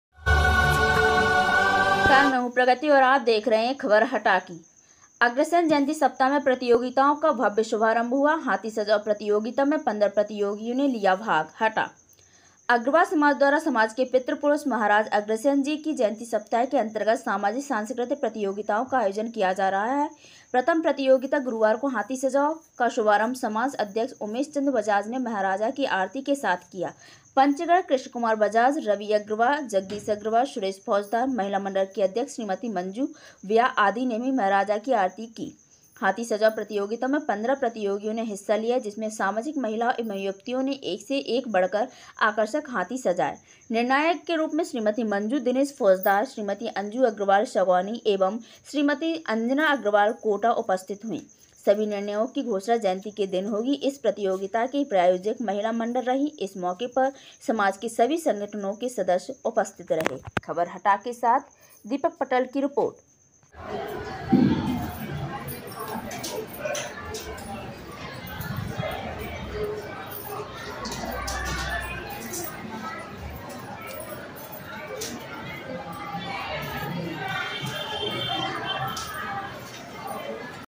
अग्रसेन जयंती सप्ताह में प्रतियोगिताओं का भव्य शुभारंभ हुआ
हाथी सजाओ प्रतियोगिता में 15 प्रतियोगियों ने लिया भाग,,,